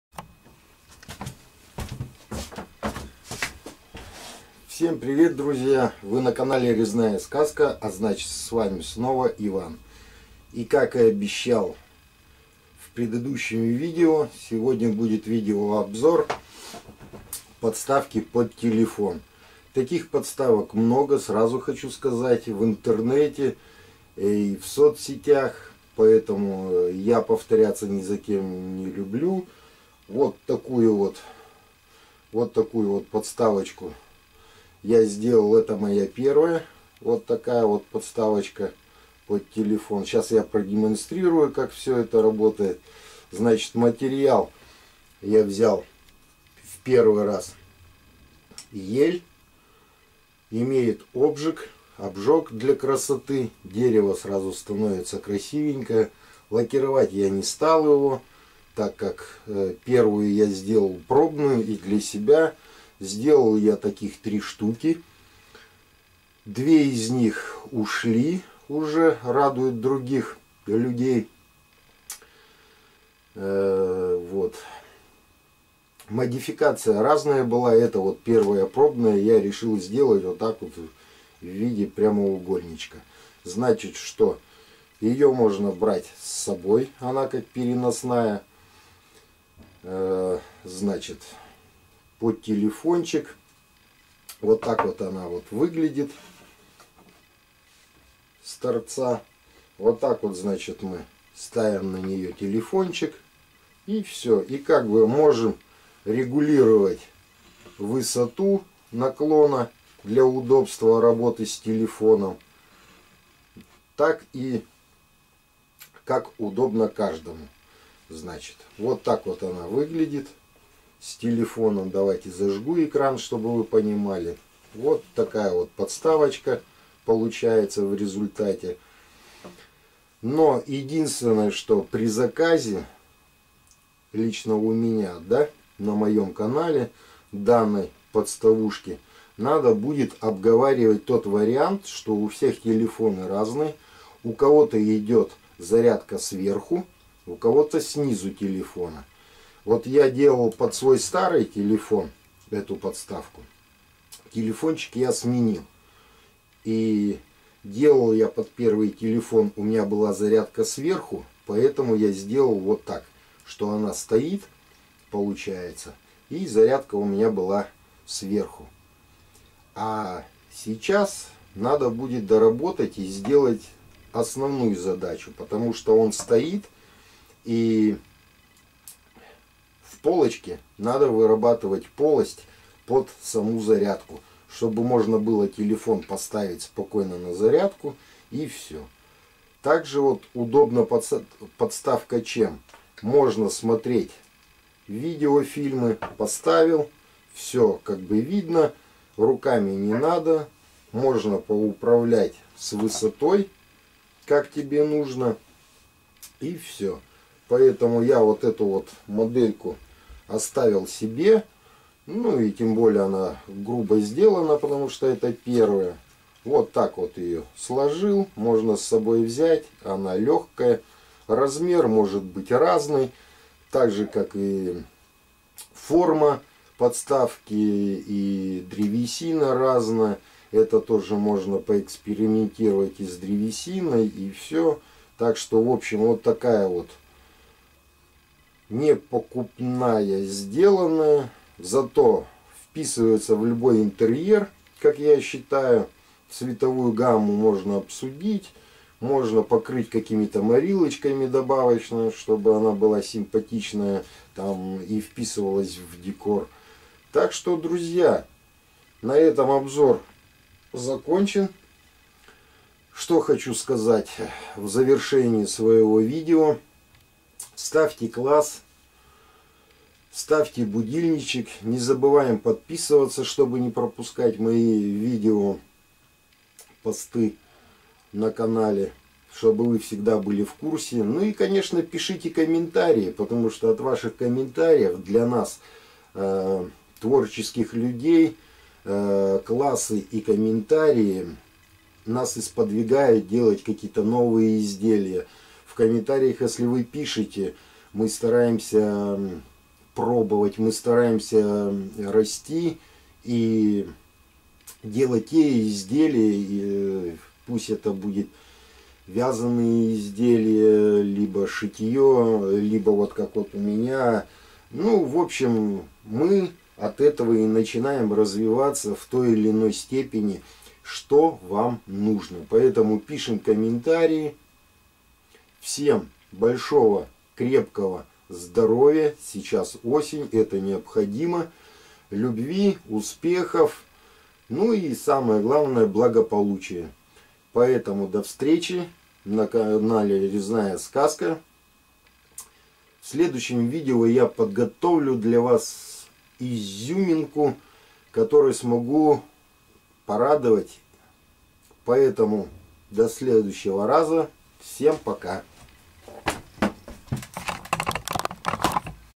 Резьба по дереву подставка своими руками из дерева для телефона
В данном видео ролики идёт разговор и обзор готовой подставке, но экспериментальной так что не судите автора своёй идеи и работы строго.

Всем мы делаем что-то в первый раз и под час не зная плюсы и минусы от начало работы и до конца, как и что получиться, даст ли дерево воплотить идею и до делать её до конца по многим факторов.

Но автор будет рад вашим идеям в комментариях, так же оценки идеи и выполненной работы, так же автор всегда рад помочь советом, с выбором древесины и просто ответить на ваши комментарии.